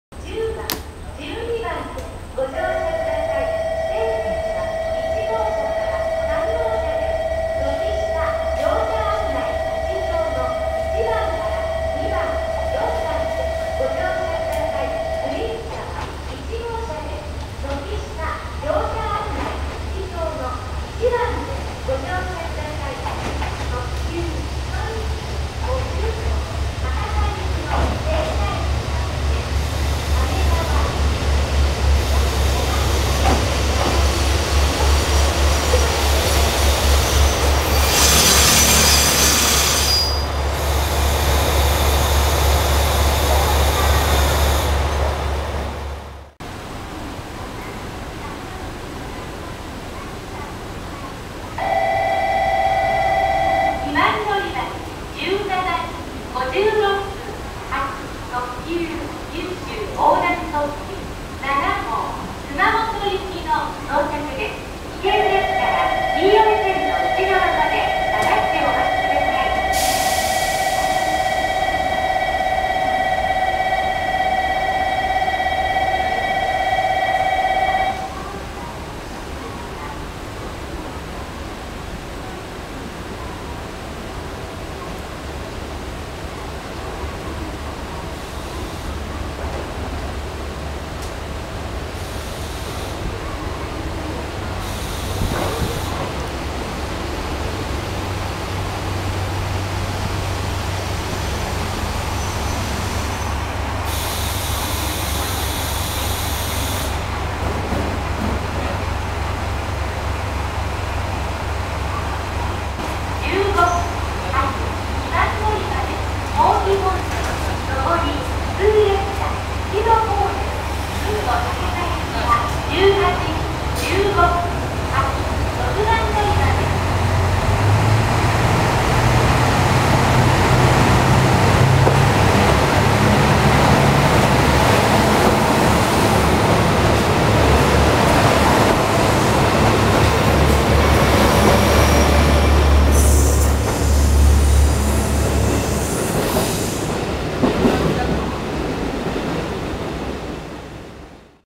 キハ185系ゆふ車 九州横断特急7号 大分駅
ここ最近、ゆふ用のキハ185系が九州横断特急の運用に入っています。赤一色の九州横断特急の中にあって、異彩を放っています。